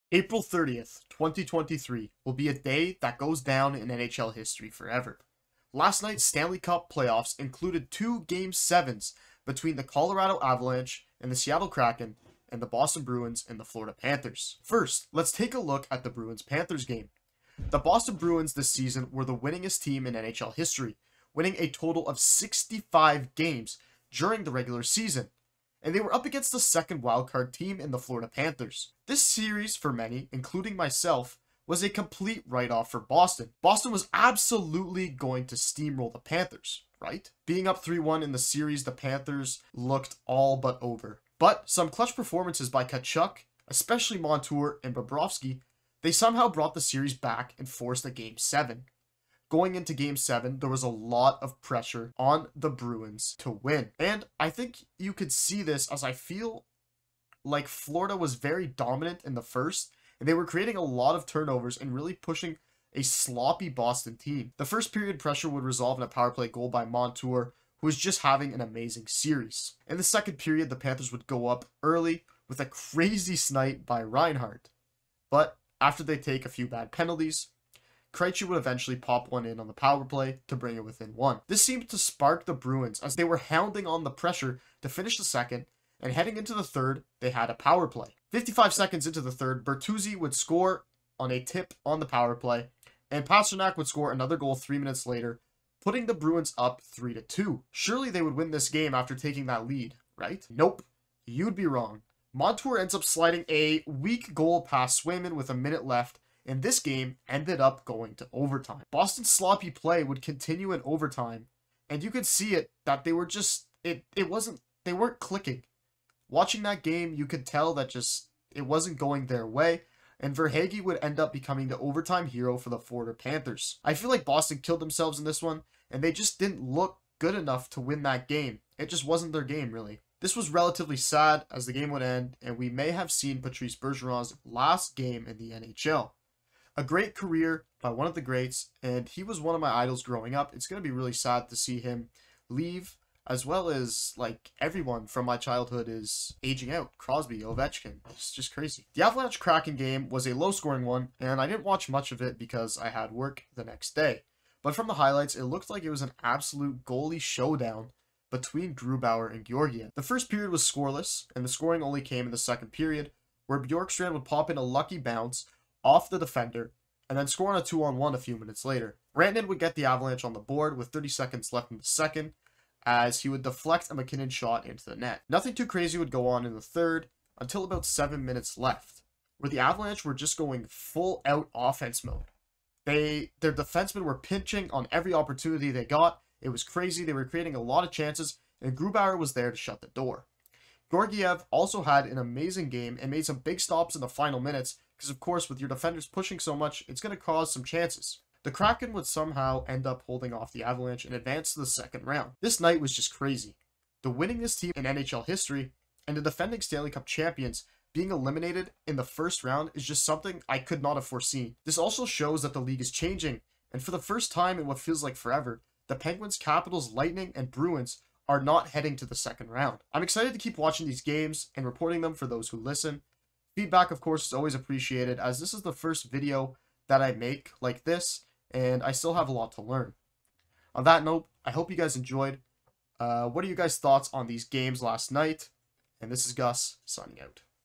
The CRAZIEST CHOKES in NHL HISTORY!
Both the Boston Bruins and the Colorado Avalanche lost there game 7 games last night. The winningest team in NHL History and the DEFENDING CHAMPS were both knocked out by Wild Card teams. In this video I give some insight and examine both of game's. What are your thoughts on the game's last night?